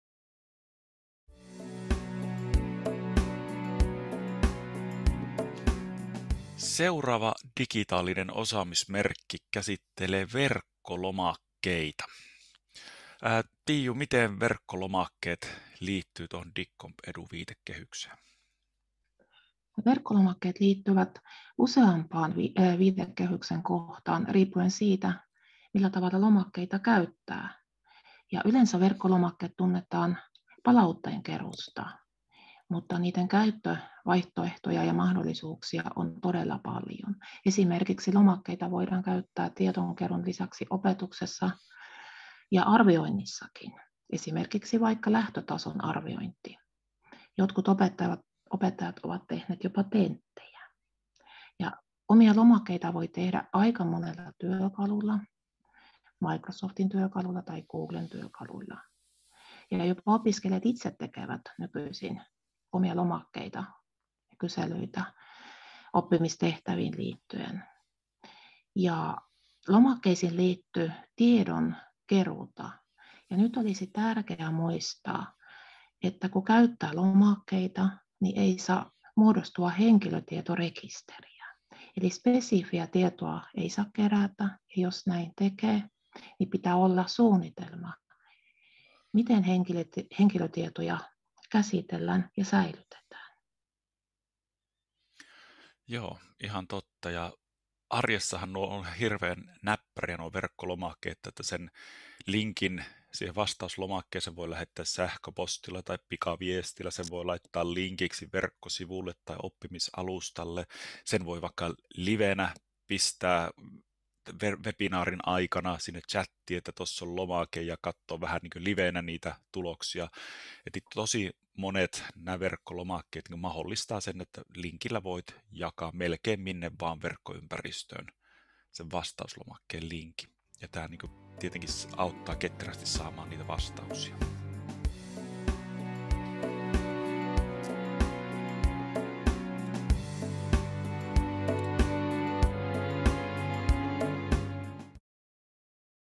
Opettajan digiosaaminen: Verkkolomakkeet
Opettajan digiosaaminen
Minipodcast: Verkkolomakkeet -osaamismerkki kuuluu opettajan pedagogiisin opintoihin. Lyhyessä keskustelussa kerrotaan esimerkkejä aiheeseen liittyvistä toimintatavoista, sovelluksista sekä siitä miten aihe liittyy DigComEdu -viitekehykseen.